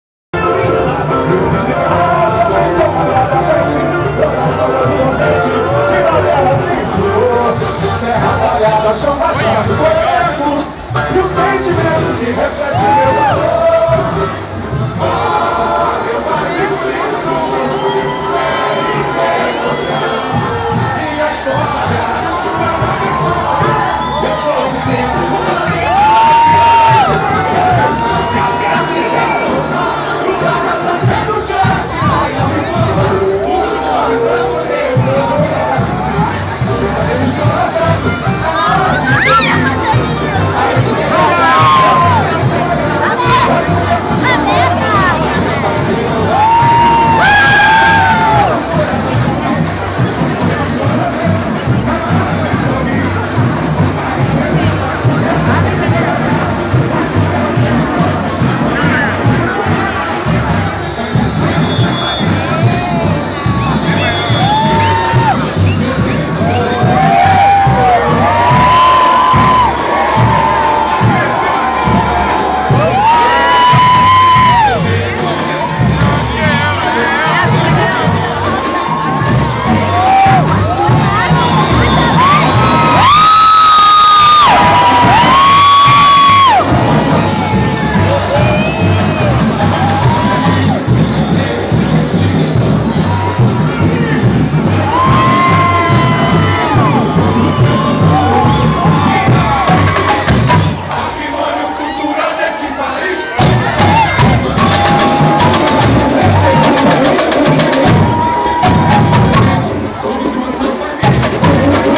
MANCHA VERDE DESFILE CARNAVAL 2023 | ANHEMBI | MANCHA VERDE PALMEIRAS
Uma parte do desfile da Mancha Verde 2023, impecável!